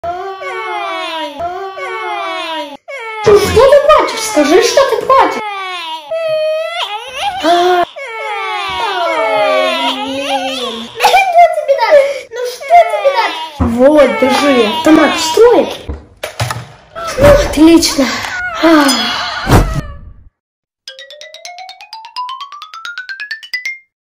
Stefan found a boy doll and pretends to be a parent - Niñera del futuro
Stefan found a boy doll and pretends to be a parent niñera extraña- Niñera del futuro